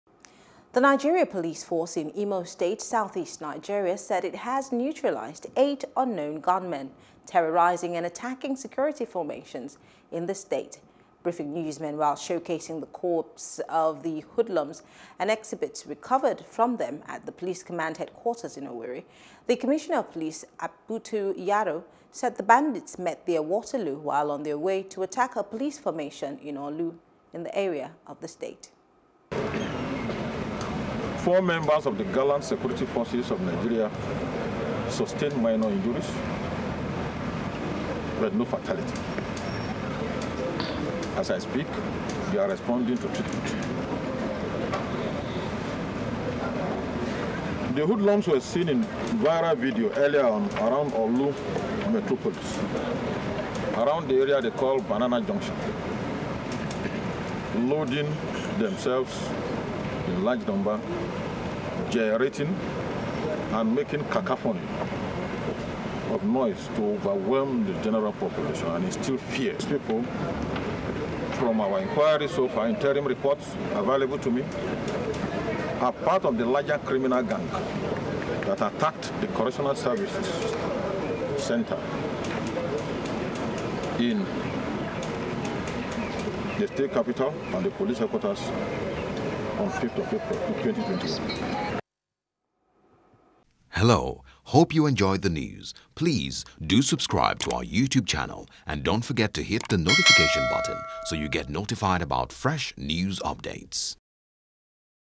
Security Agents Kill 8 Unknown Gunmen Who Attacked Correctional Facility In Owerri | NEWS
The Nigeria police force in IMO state, south east Nigeria, said it has neutralized 8 unknown gunmen terrorising and attacking security formation in the state